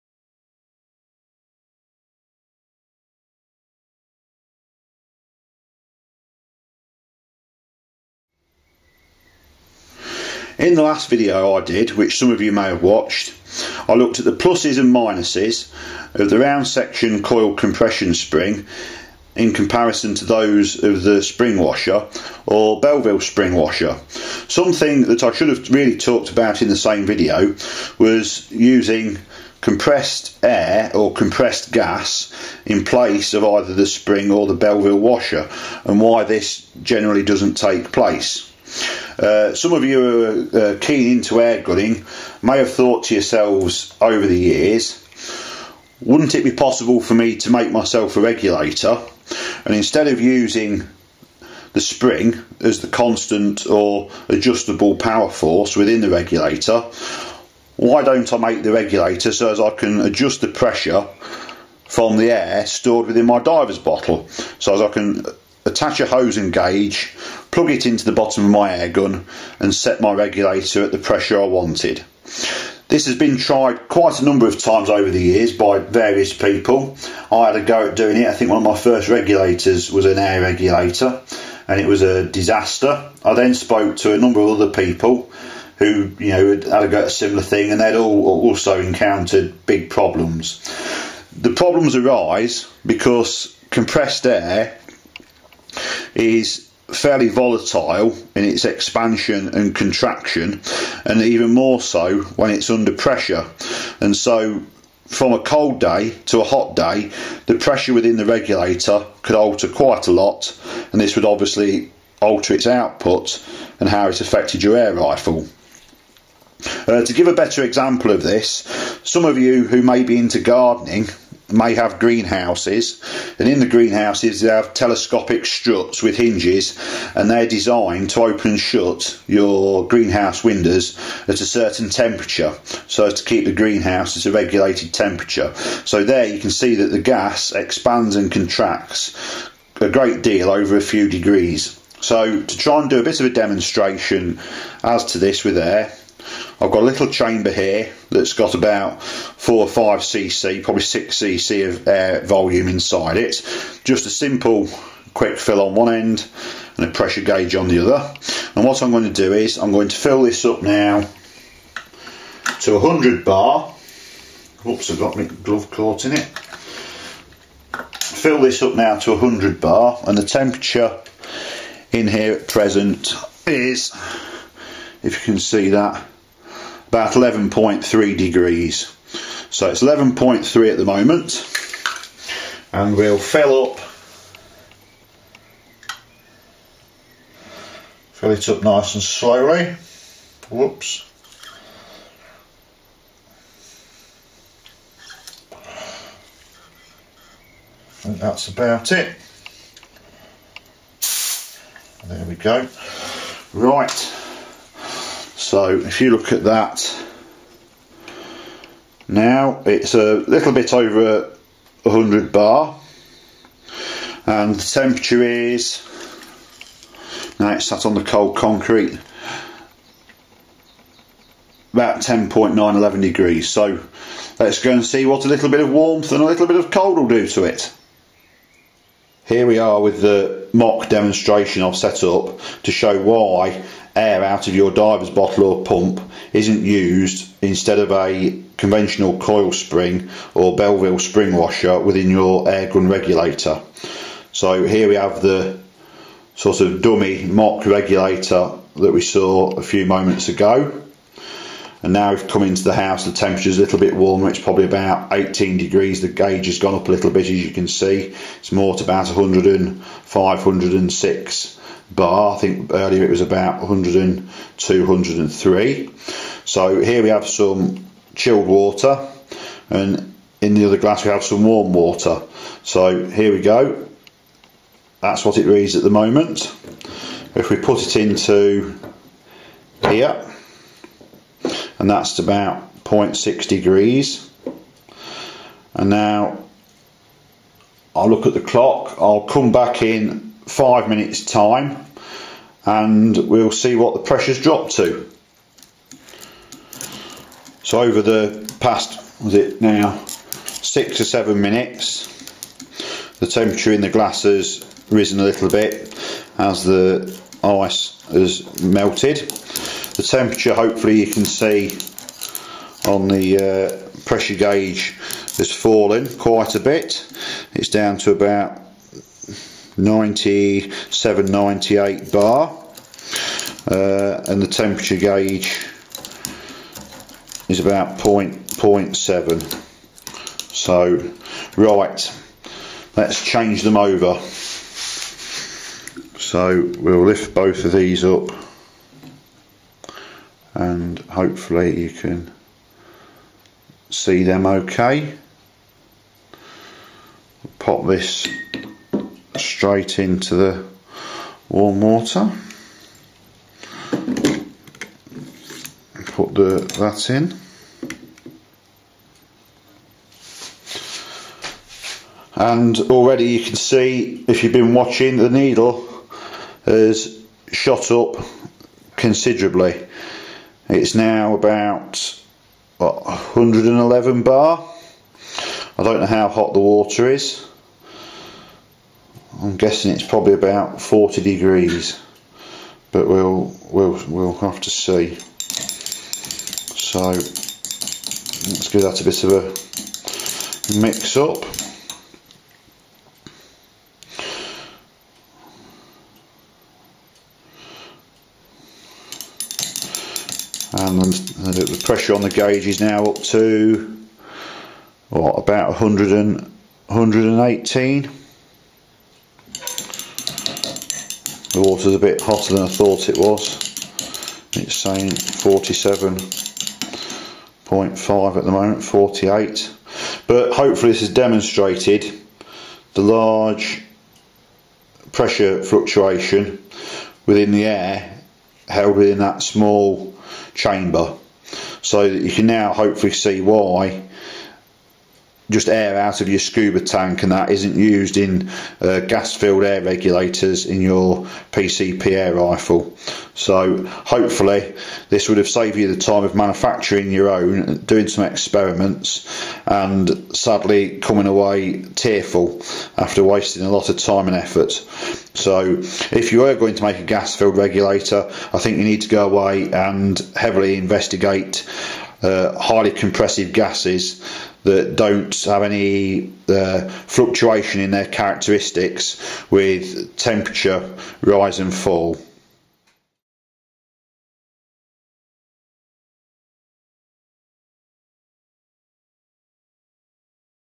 Lane Regulators - Pt. 3-3 Coil Spring v Spring Washers v Gas Spring.
One of the elements that make up a regulator of any sort, is a 'Constant Set Force'. This is usually supplied by a Coil Compression Spring or a Spring Washer / Bellville Spring Washer.

Some of you may wonder why this force cannot be simply supplied by the stored energy in compressed Air / Gas i.e. a GAS SPRING. For you have a Pump or Scuba Bottle at your disposal, why not make a gas filled chamber instead of a chamber filled with a Coil Spring or Belleville Spring Washer Stack ?

Simply the answer is that Highly Compressed Gases are in general to volatile to be used, as they are very reactive to temperature change, expanding & contracting wildly.

Example as shown in the video: You may go out at 4 am in the morning to hide up & the chill in the breeze is  +1 / -1c & the Regulator Gas Pressure drops to 97 bar, but then come lunch time in the heat of the midday sun the rifle maybe in direct sunlight & the black metal finish makes the metal work hit 40c & so the Regulator Gas Pressure is now 120bar.

This large Regulator Gas Pressure Alteration causes serious Power Issues :-(